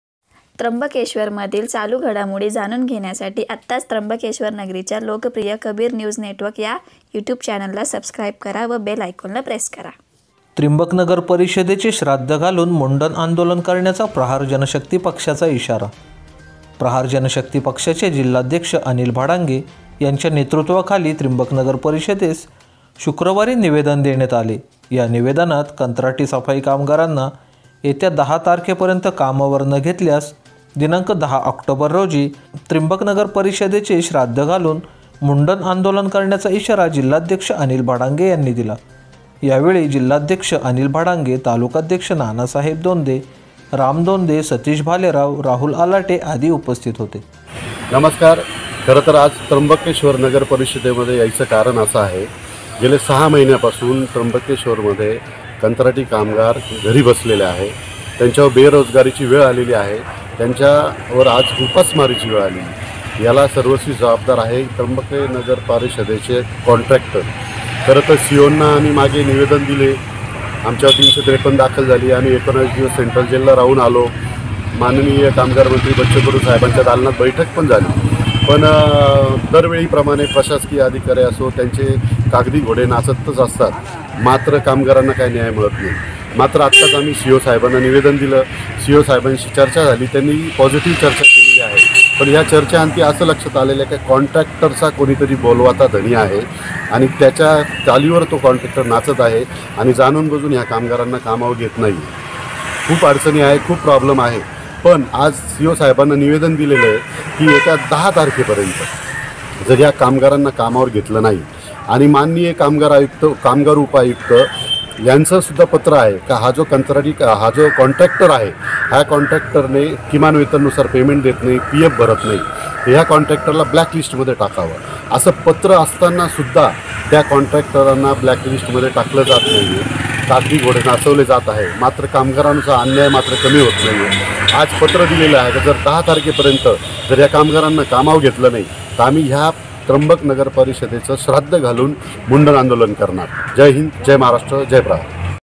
त्रिंबक नगरपरिषदेचे श्राद्ध घालून मुंडण आंदोलनाचा प्रहार जनशक्ती पक्षाचा इशारा
मागण्या मान्य न झाल्यास १० तारखेला करणार आंदोलन